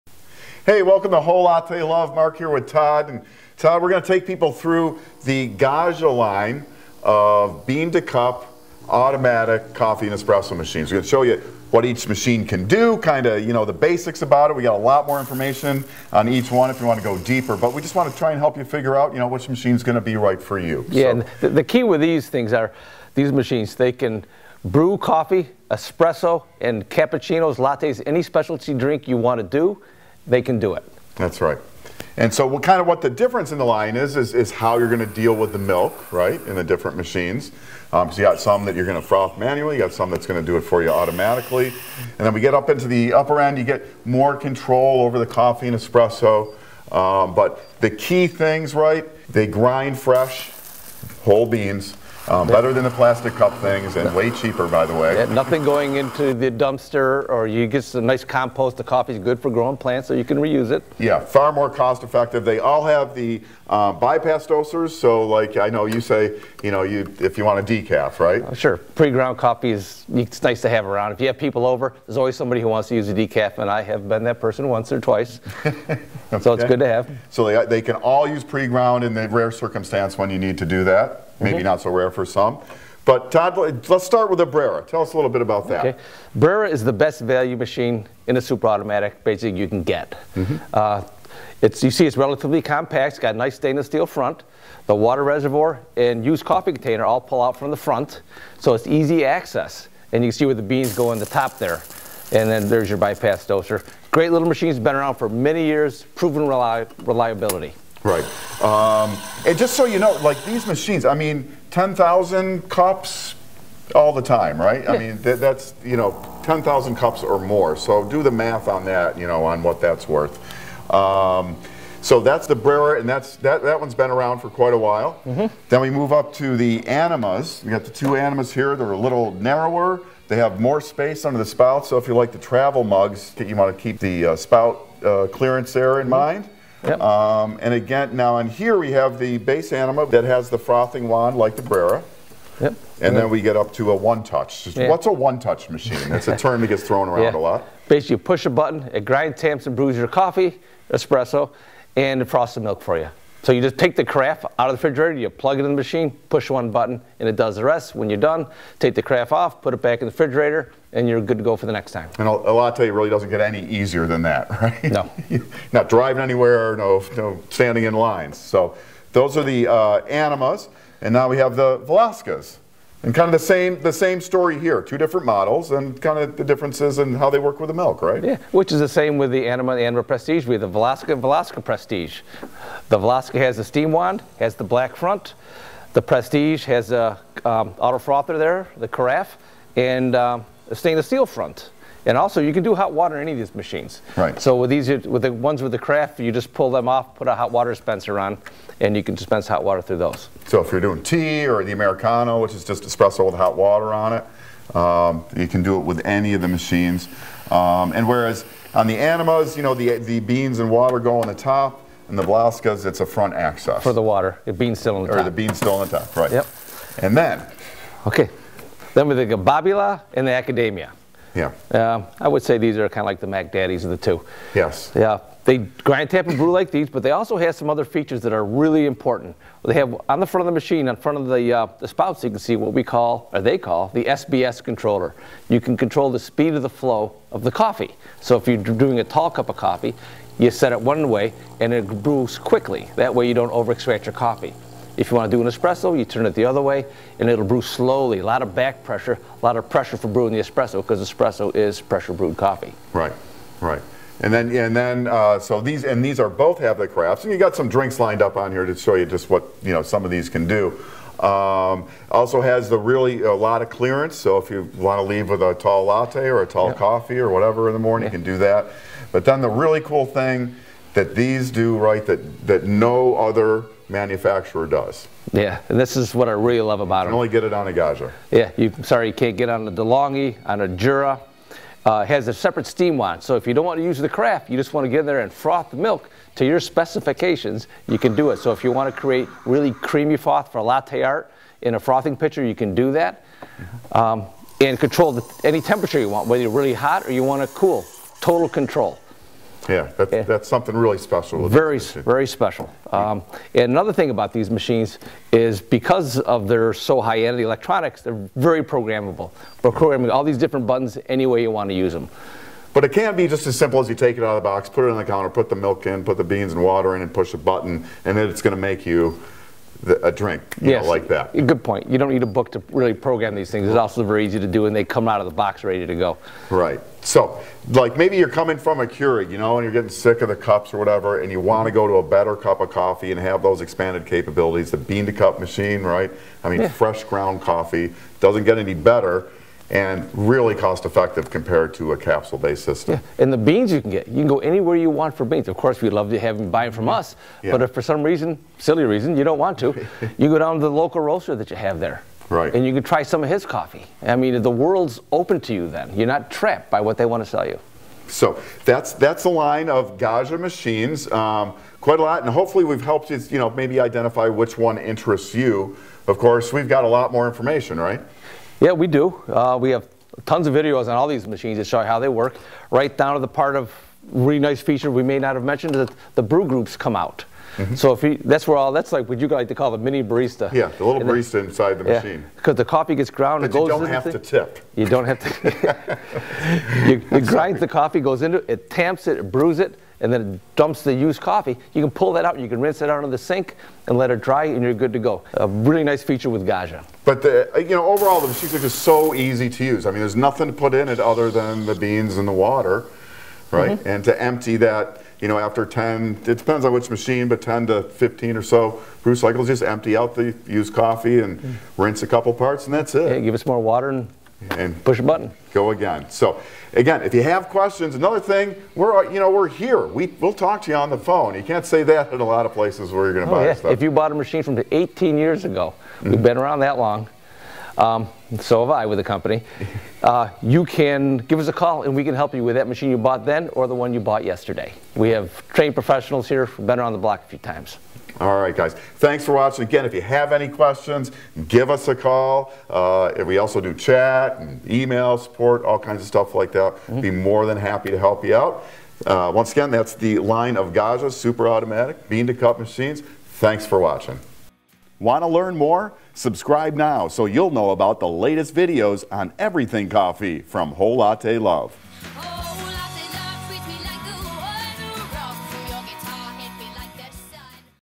Automatic Coffee, Latte, Espresso from Gaggia Bean to Cup Machines
Compare and review the full lineup of automatic bean to cup coffee machines from Gaggia. Enjoy the best lattes, cappuccino, coffee, espresso and more at the touch of a button made from freshly ground whole bean coffee. Easy to use and so convenient, these machines blow away coffee cup/capsule machines in flavor, capabilities and cost per cup!

Learn more about Gaggia bean to cup automatic coffee machines at Whole Latte Love